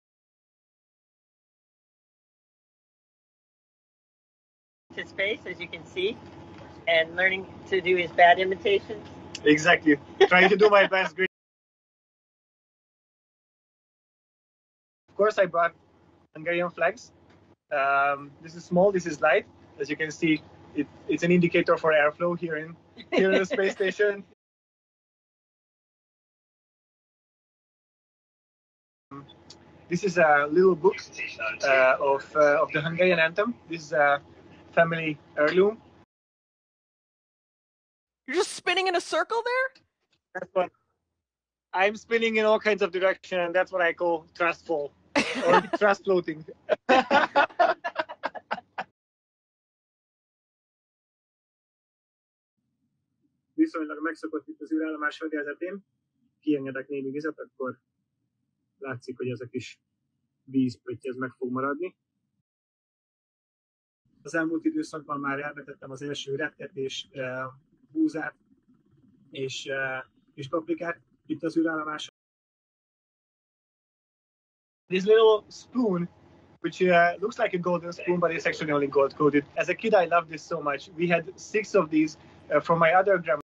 Tibi jön haza
Holnap 2025 július 14-én magyar idő szerint 13 órakor  indul Tibi haza a Nemzetközi Űrállomásról.  Másnap szállnak le az óceánra, nézzétek a közvetítéseket. 13 órakor kezdődnek.